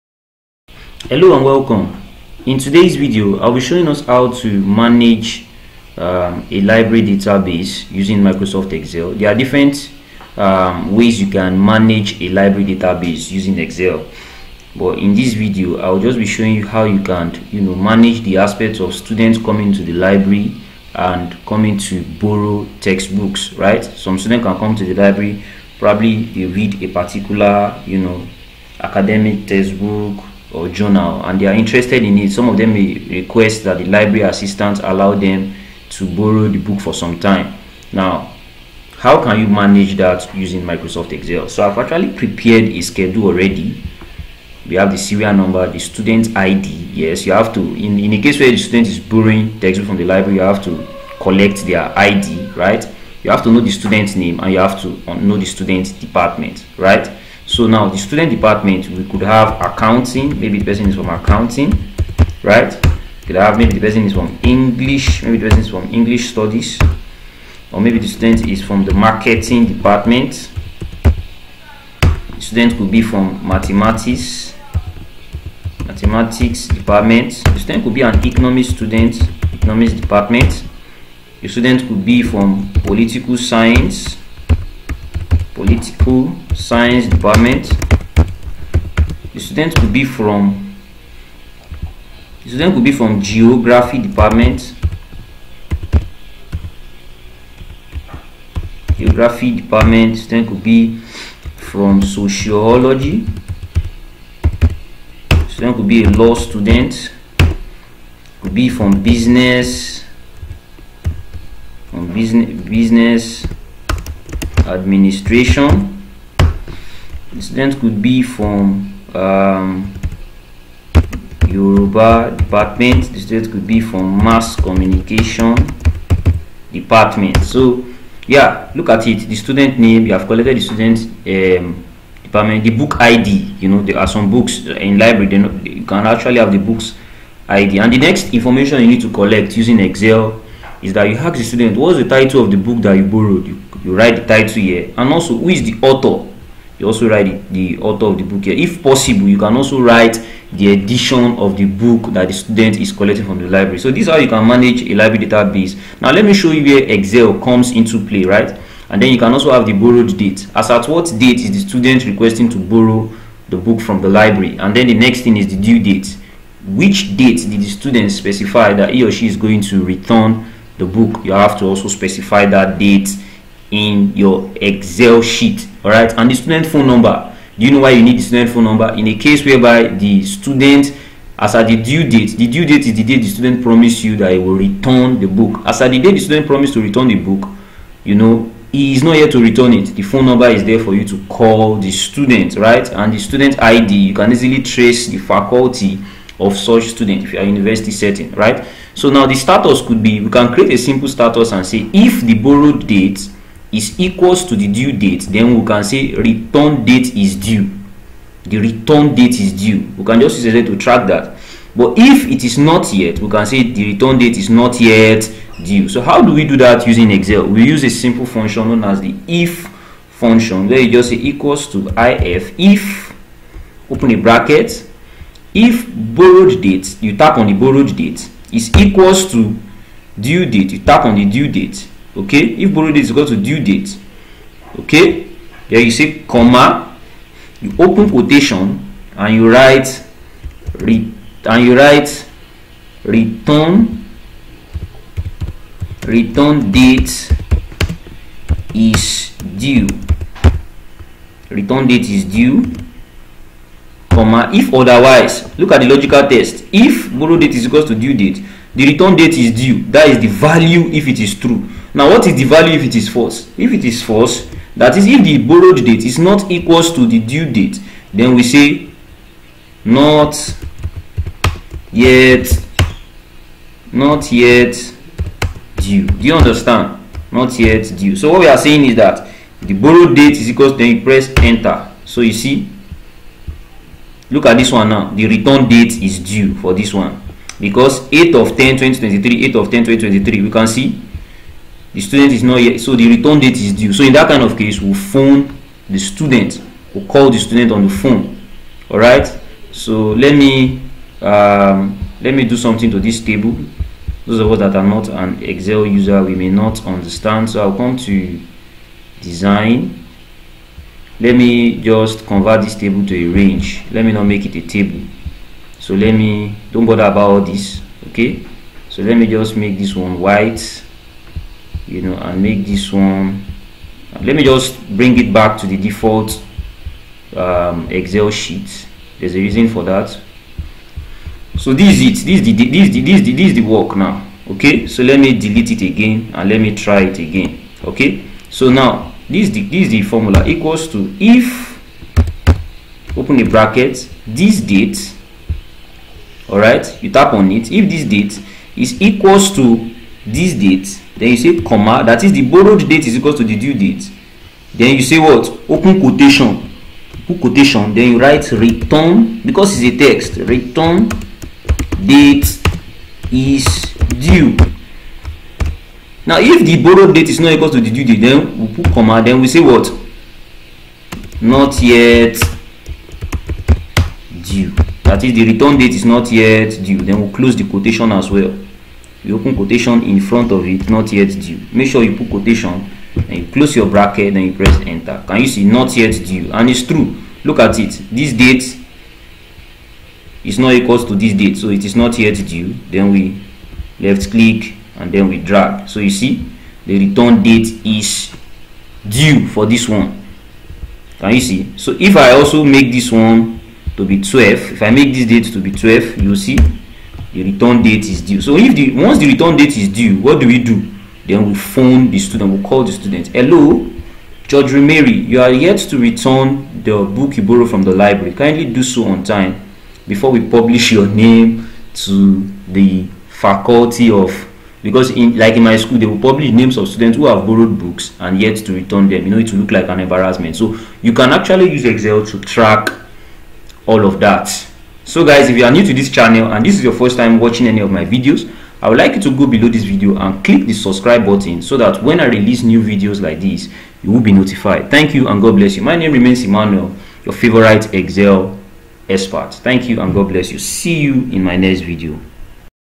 How to Create a Library Management System Program in Microsoft Excel
In this video tutorial I will be showing us how to manage a Library data base. Most especially being able to track books borrowed from the Library. This particular activity can be tracked using Microsoft Excel.